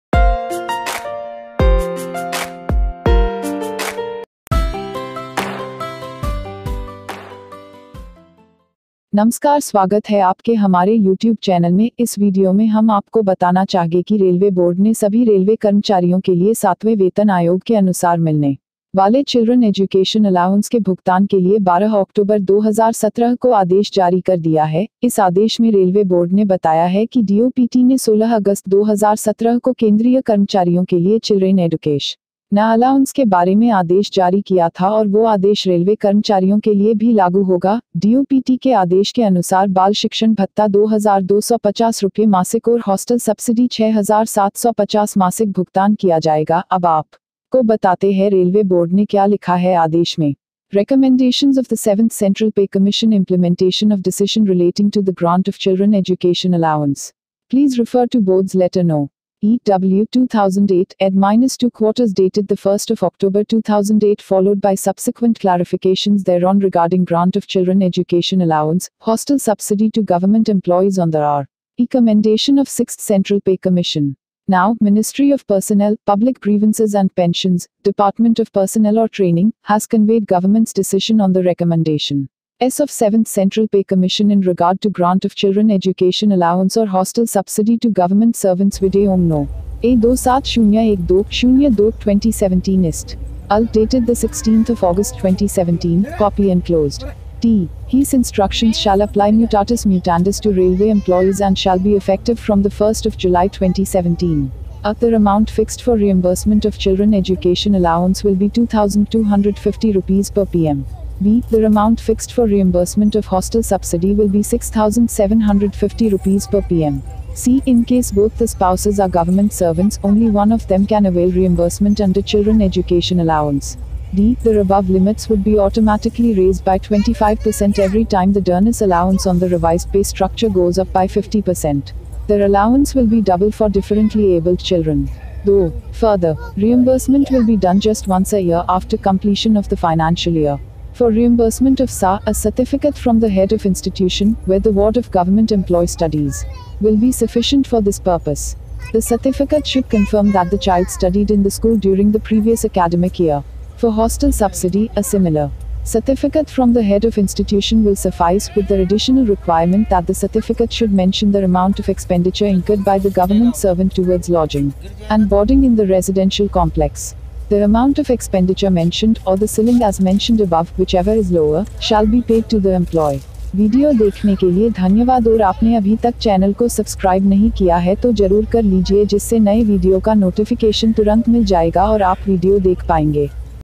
7th Pay Commission रेलवे कर्मचारियों को Children Education Allowance के लिए Railway Board order
Recommendations of the Seventh Central pay Commission – Implementation of decision relating to the grant of Children Education Allowance. 7thpay commission latest news, children Education Allowance for railway employees, education